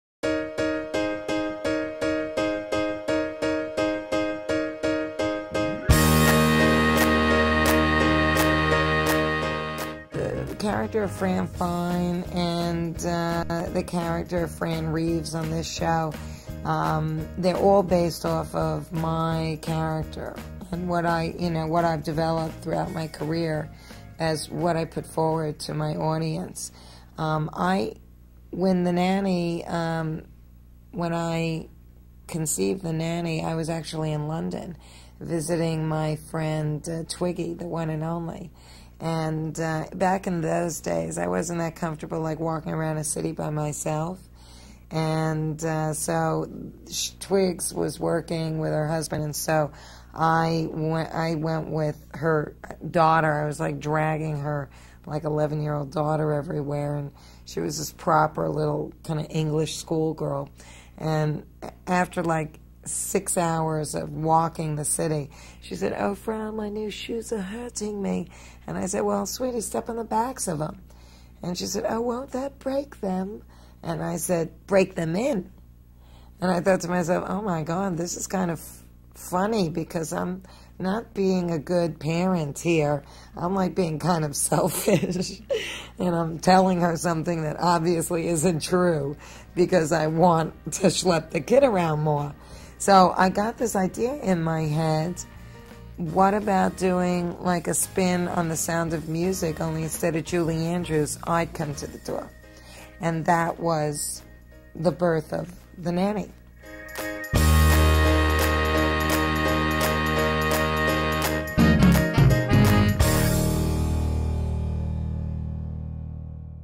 Fran Drescher Creating the Nanny
Fran Drescher reveals the story of how she created her hit show THE NANNY